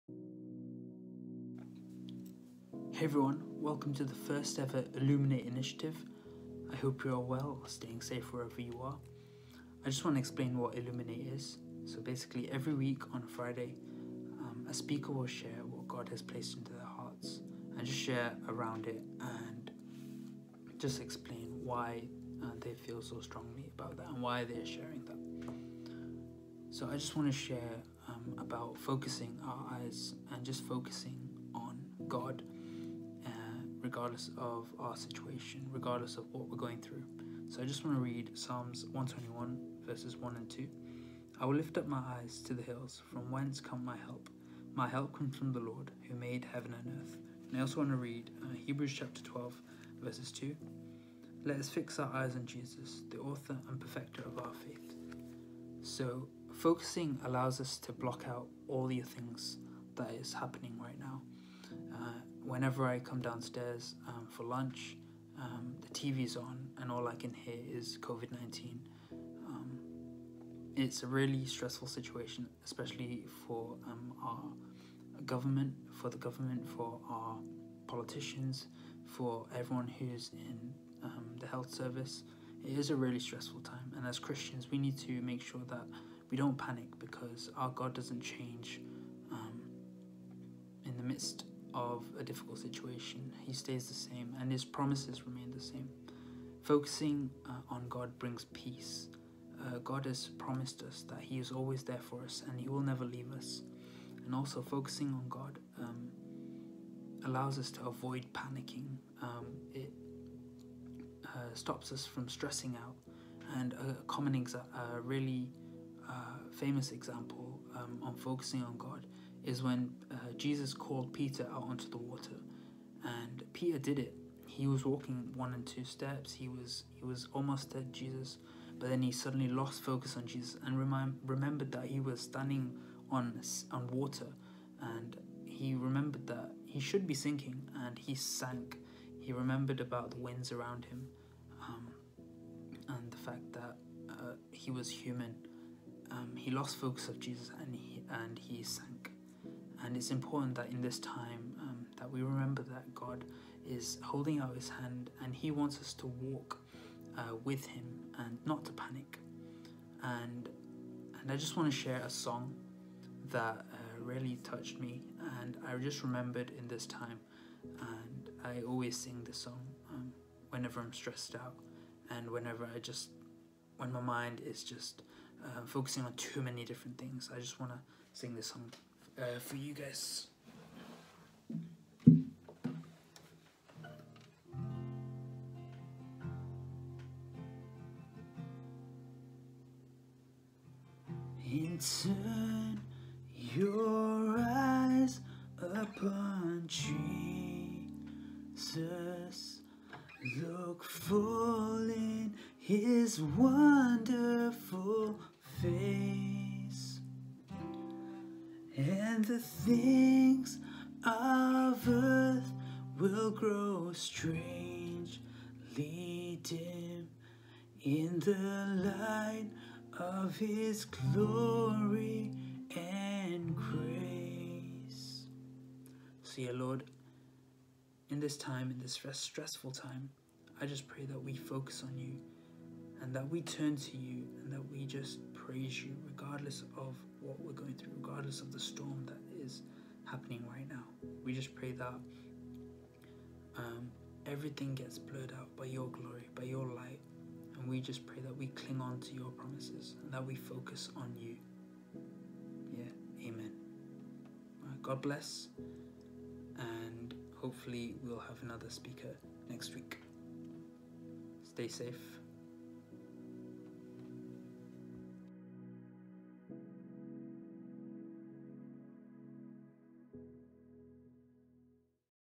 Illuminate Initiative: Focus Your Eyes
Welcome to the first Illuminate Initiative!
This is a chance for a speaker to share a verse or thought that God has placed in their heart.
This week, Benji, brings the thought. We hope you are blessed through this!